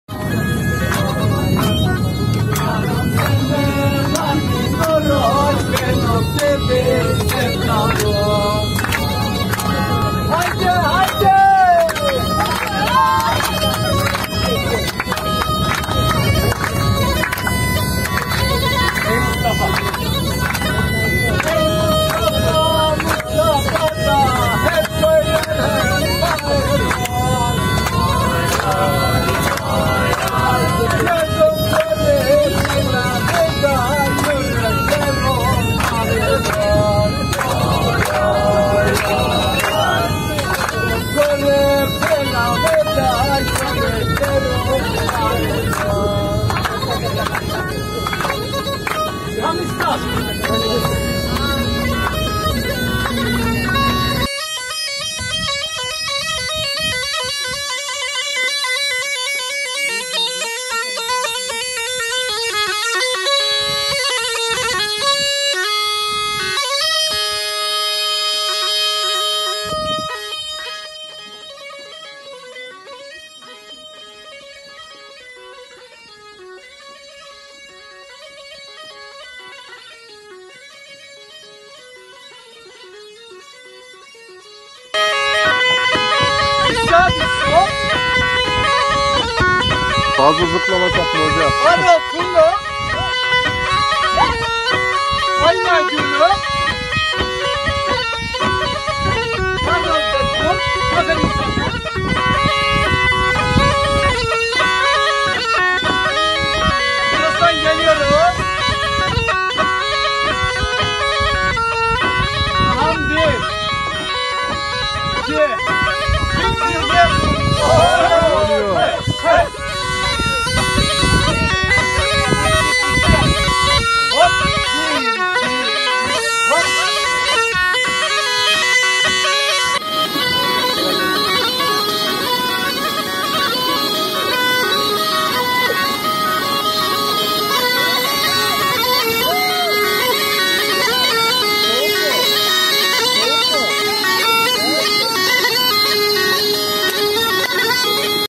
Tulumlu Doğa Gezisi
Karadeniz İlleri Kültürünü Koruma  Sosyal Yardımlaşma ve Halk Oyunları Eğitim Derneği Ankara'dan Kastamonu'ya Kültür ve Doğa Gezisi düzenledi.